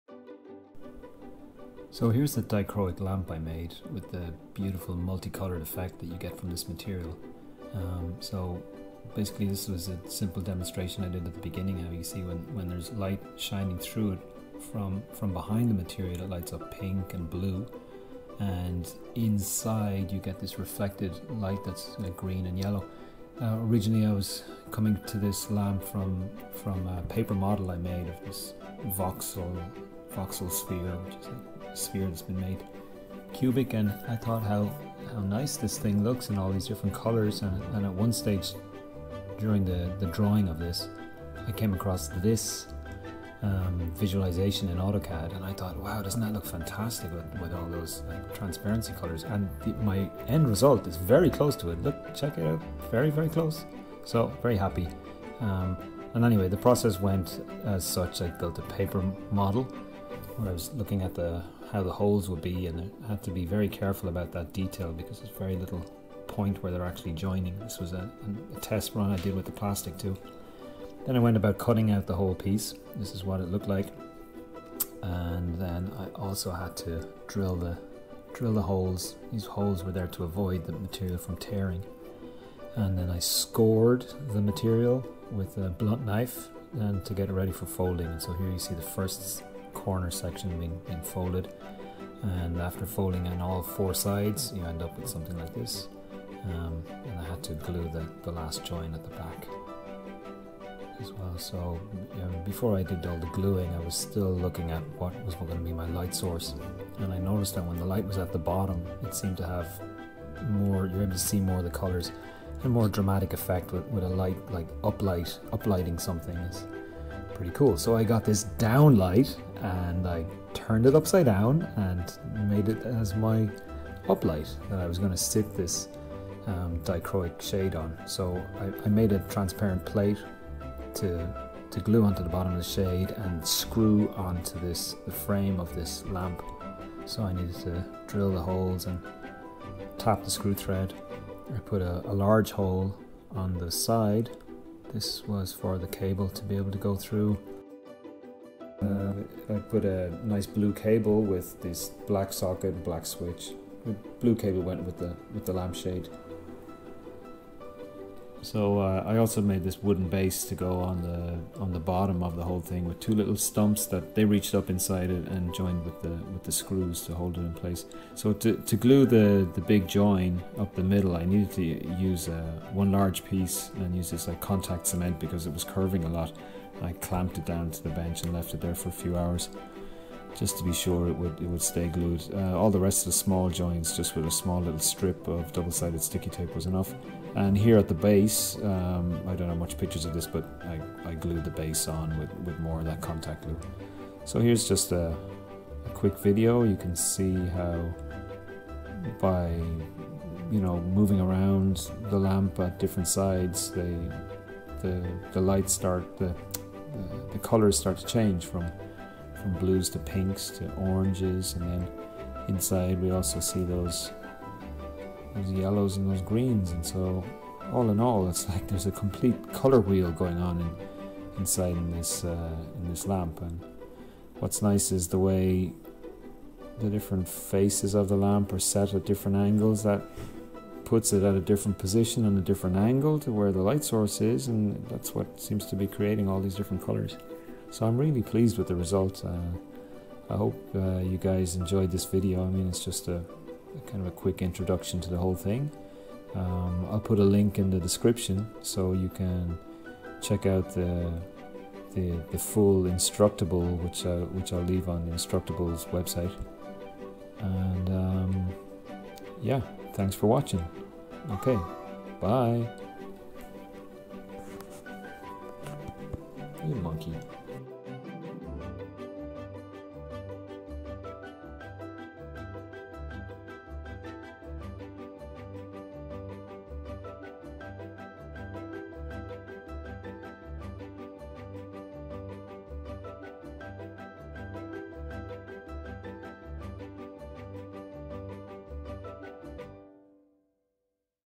DIY Dichroic Lamp
Hello there this video is a quick summary of how I made this lamp. There are more complete instructions, drawings to download and more photos to be seen on the instructables website.